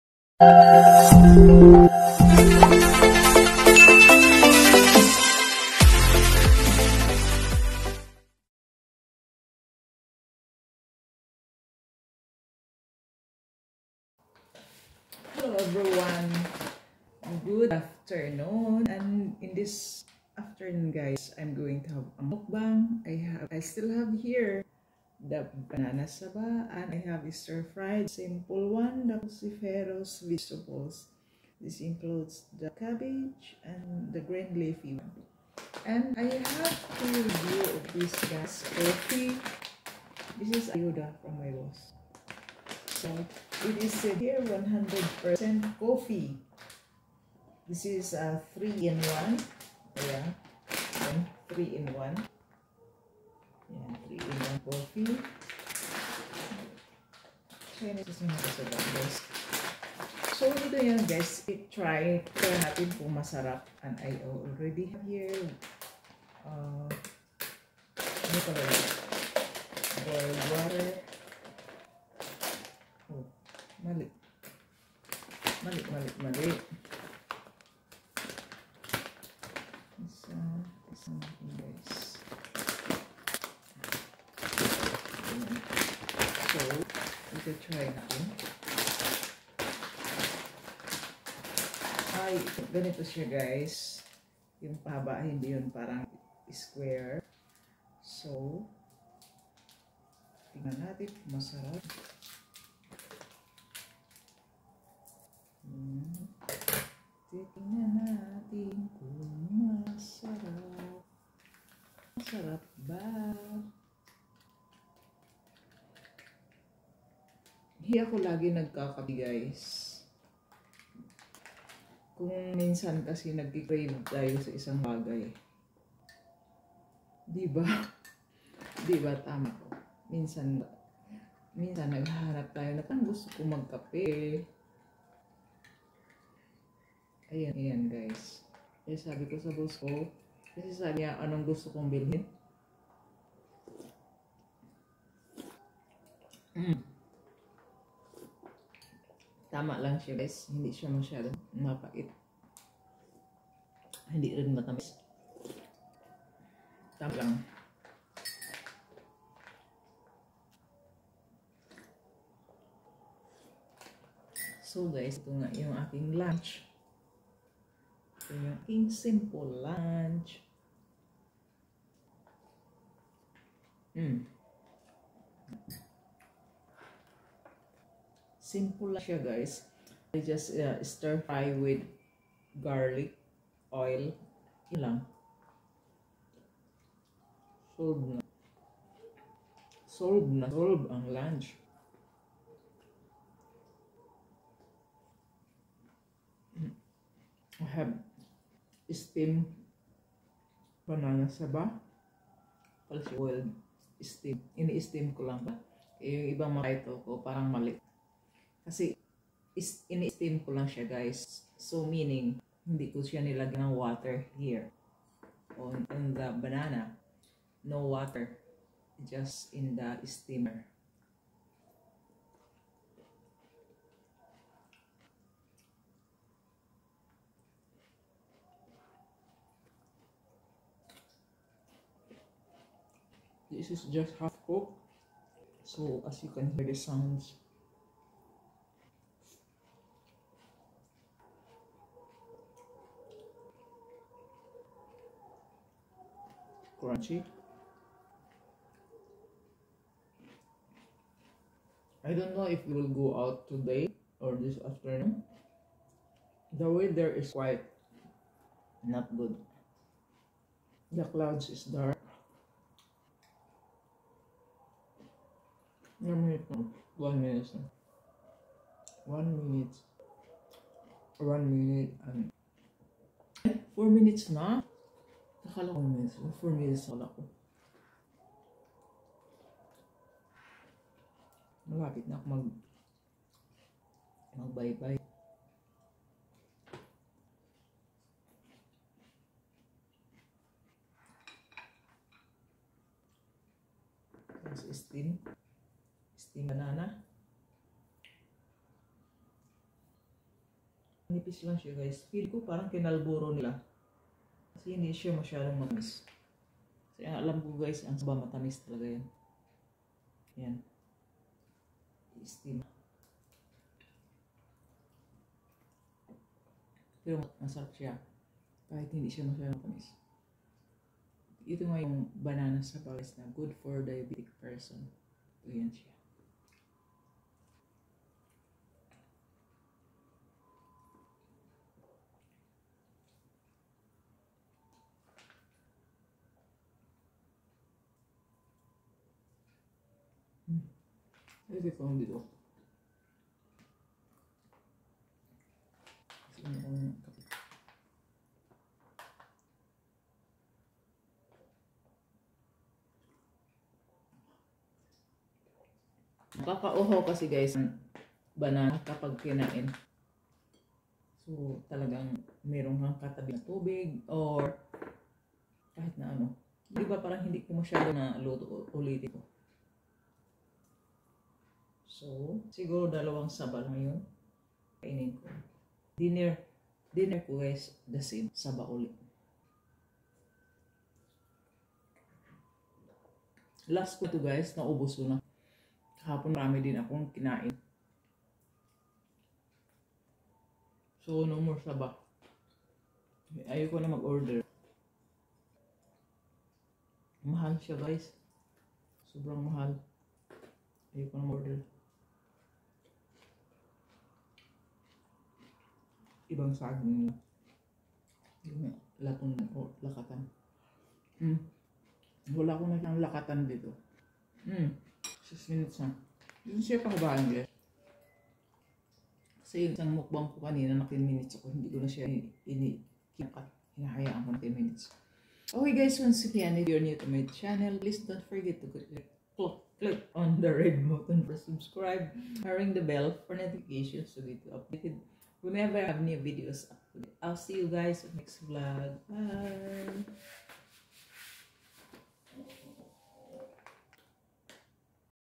Coffee ☕ 3n1, Banana 🍌 Sabá,& Cruciferous Veggies Stir Fry My Lunch Mukbang 😋
In this video I am going to share my lunch mukbang I have here 3 in 1 coffee ☕,  steamed banana 🍌 Sabá  and cruciferous vegetables stir-fry.